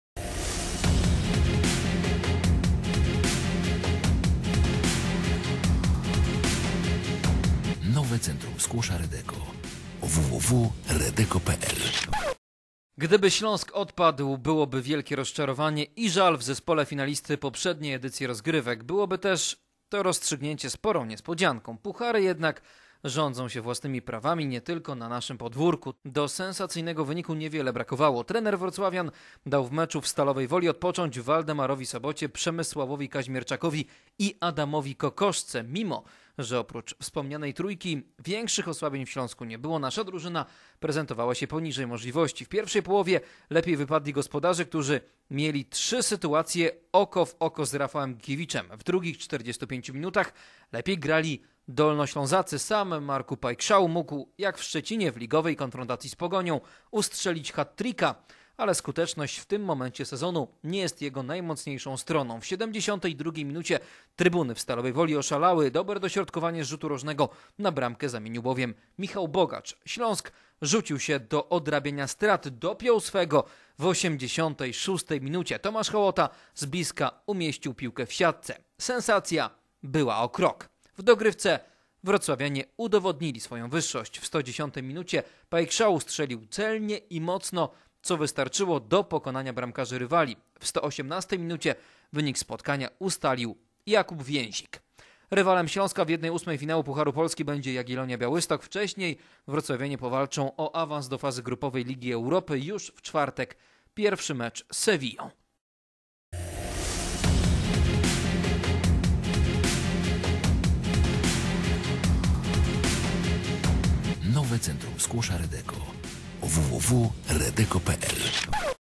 Śląsk Wrocław w kolejnej rundzie Pucharu Polski
Sensacja było blisko, wrocławski faworyt jednak się uratował .Piłkarze Śląska awansowali do 1/8 finału Pucharu Polski. Pokonali po dogrywce w Stalowej Woli Stal 3:1. Teraz zespół Stanislava Levego szykuje się do dwumeczu kwalifikacji Ligi Europy z Sevillą.